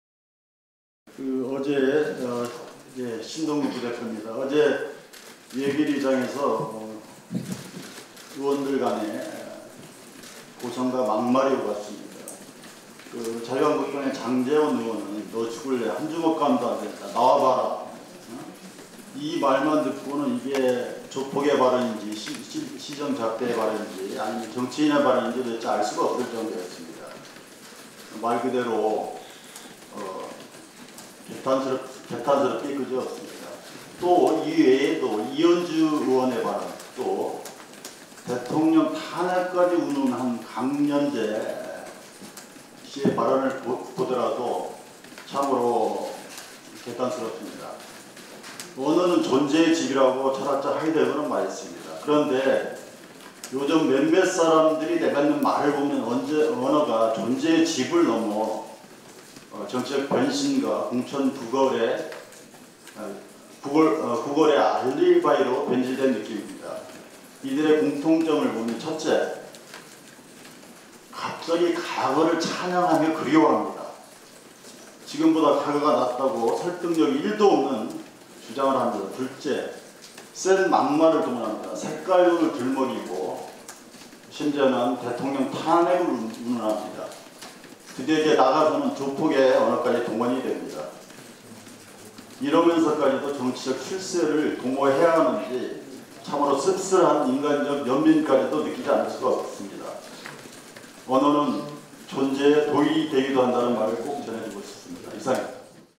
신동근 의원 "장제원 발언, 조폭인지 시정잡배인지 모르겠다"
11월 6일 국회에서 진행된 더불어민주당 원내대책회의에서는 신동근 의원이 전날 예결위에서 있었던 장제원 의원의 발언에 대해 언급했다.